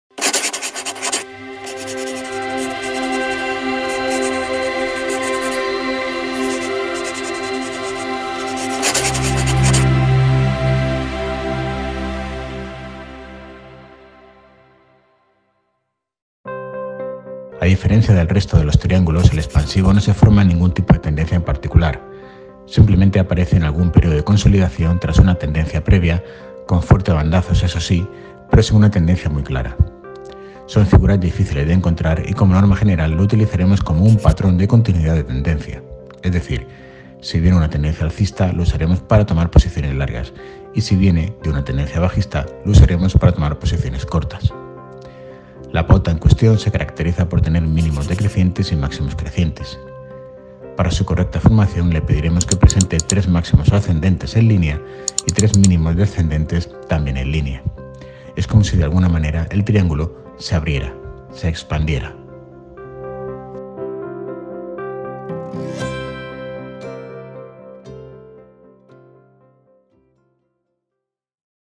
26. El triángulo expansivo - Aprende con Bolsamanía
Se trata de una pauta difícil de encontrar, que se caracteriza por tener mínimos decrecientes y máximos crecientes.